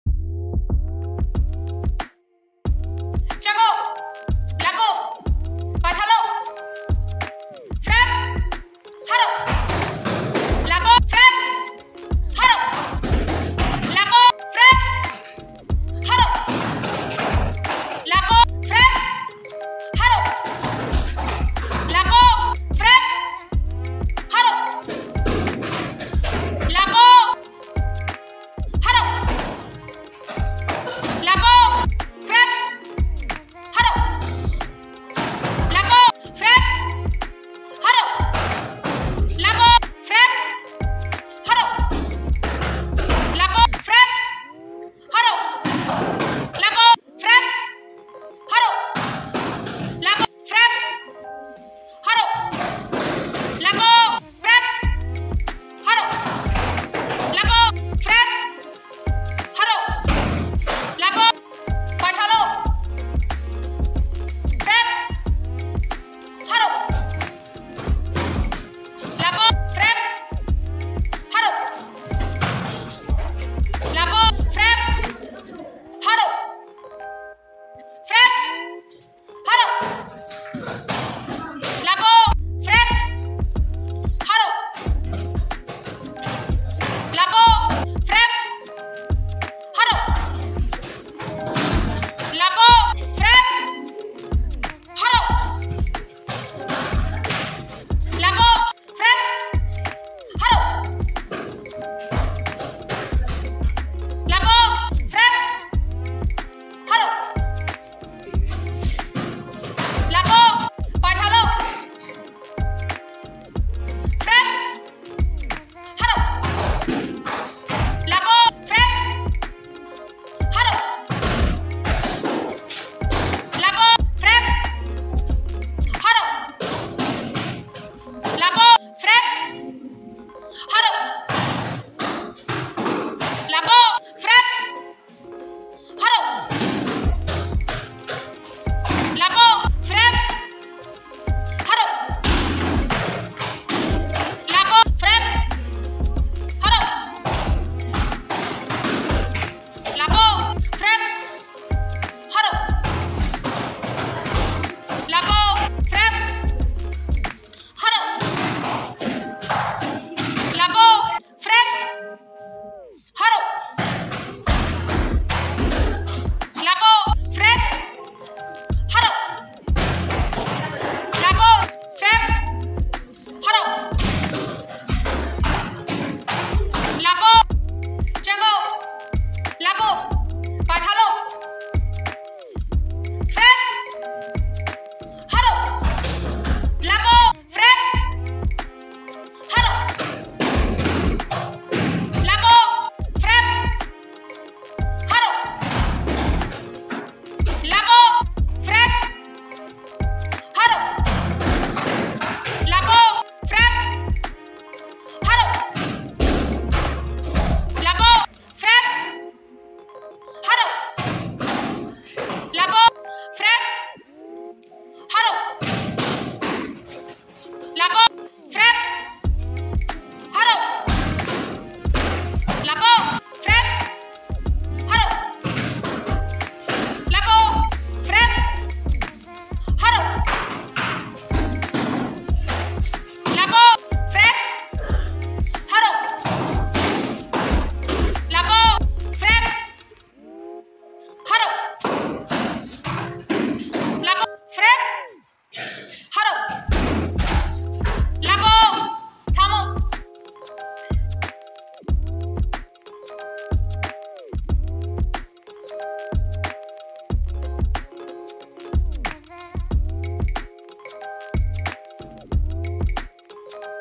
CLASS 5TH ACTIVITY : MARTIAL ARTS (THANGA TA)
CLASS 5TH
ACTIVITY : MARTIAL ARTS (THANGA TA)
Learning the art of Thanga Ta in class 5th has been an amazing experience! 🥋